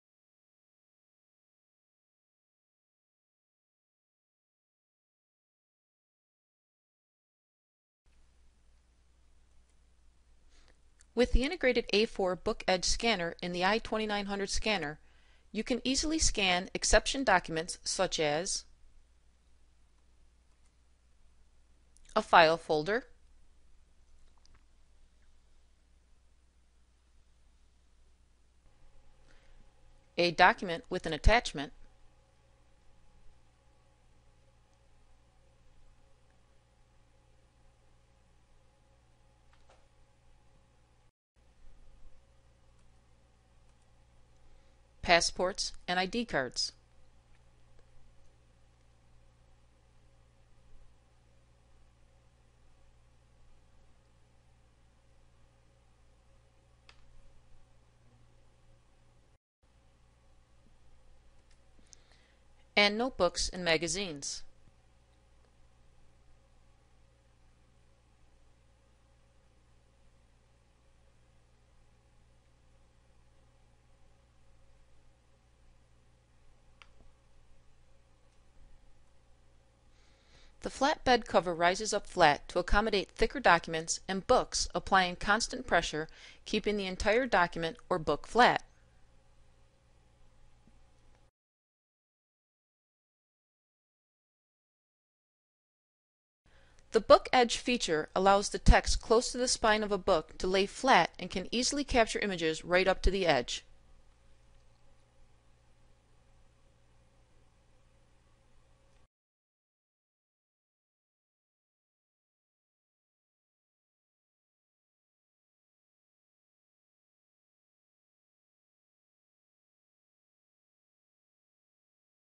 Kodak i2900 Scanner with integrated Flatbed
This video shows the Kodak i2900 with integrated Flatbed and what types of documents the Flatbed can scan